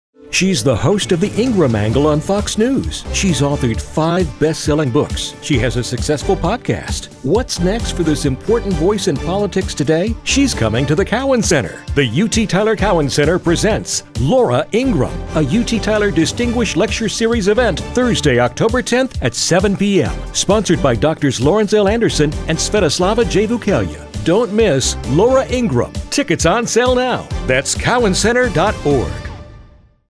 Laura Ingraham A UT Tyler Cowan Center Distinguished Lecture | October 10
Laura Ingraham
Thursday October 10, 2019, 7:00 PM
UT Tyler Cowan Center

Laura Ingraham is the host of “The Ingraham Angle” on Fox News, and an author of five best-selling books, including her most recent page-turner Billionaire at the Barricades: The Populist Revolution from Reagan to Trump. Her new three-times a week podcast is available on PodCastOne or iTunes. As a mother of three, a former litigator and Supreme Court law clerk, she brings a unique and incisive perspective to the political and cultural issues of the day.

Drs. Lawrence L. Anderson &
Svetislava J. Vukelja